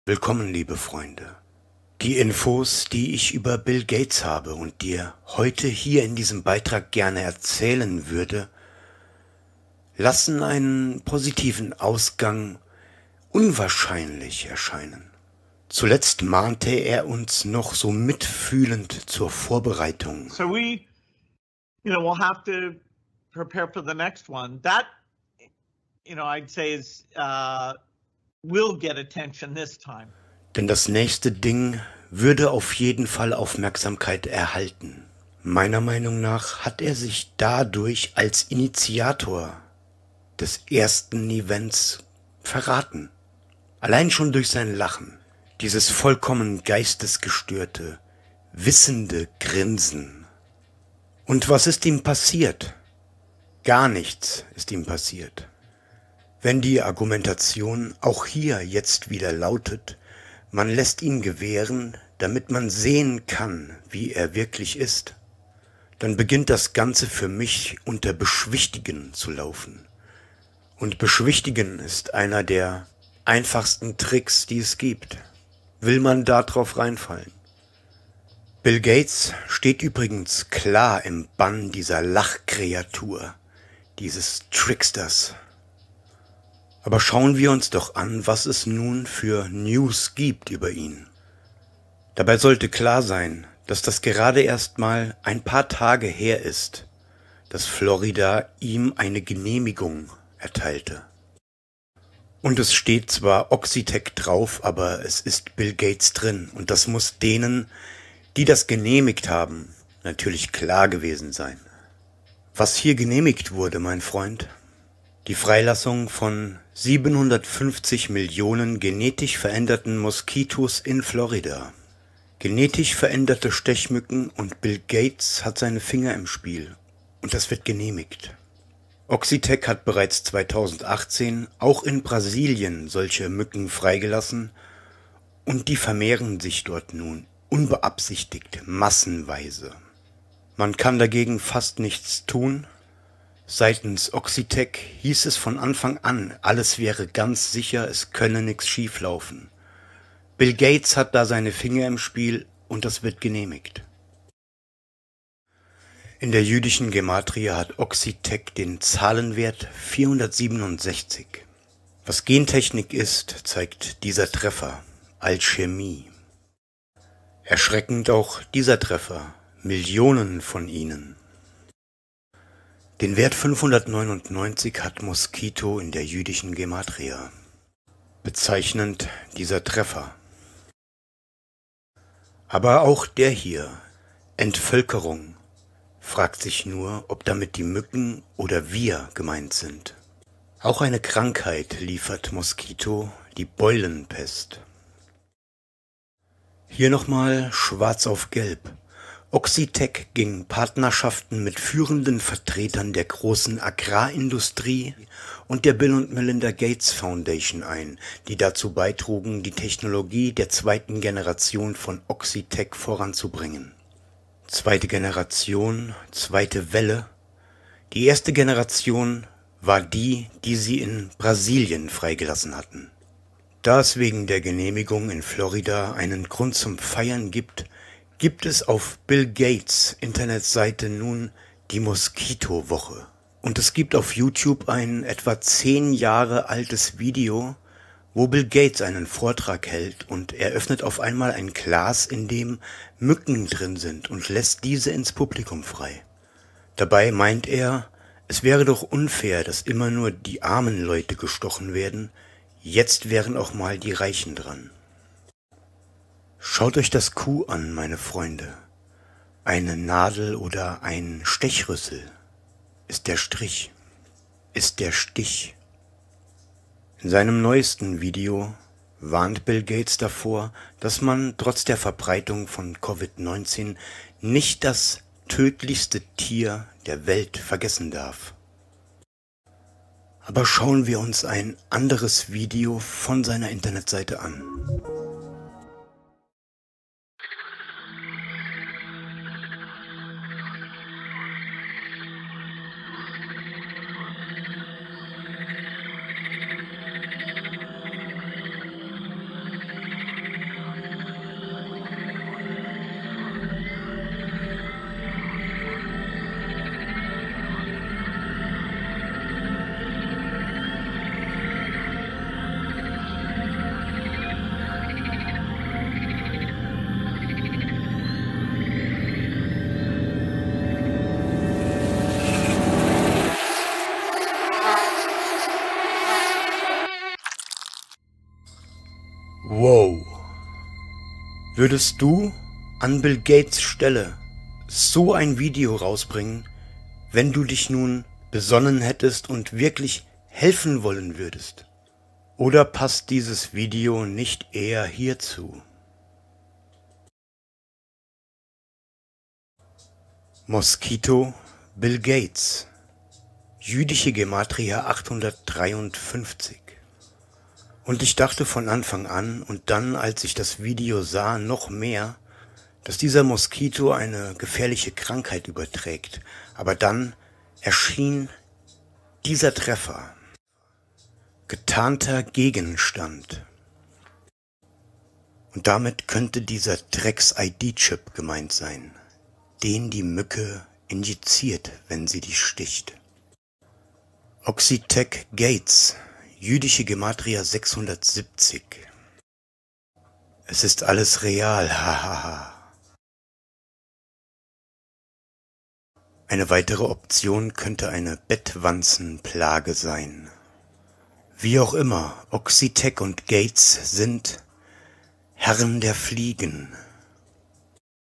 Bill Gates ➤ Herr der Fliegen (und Mücken)
Offenbarung 9:10
Und sie hatten Schwänze gleich den Skorpionen mit Stacheln; und in ihren Schwänzen liegt ihre Macht, die Menschen fünf Monate lang zu schädigen.

Bill Gates lässt 750 Millionen genetisch veränderte Mosquitos auf die Menschheit los. Die Mücken wurden von einem Unternehmen namens Oxitec genetisch modifiziert, damit diese angeblich keine Krankheiten mehr übertragen. Da können wir uns natürlich 100% drauf verlassen.

Original-Video (dancing with demons): "Bill Gates Der Herr der Fliegen lässt 750 Millionen GMO Stechmücken in Florida frei Zweiter Wahnsinn"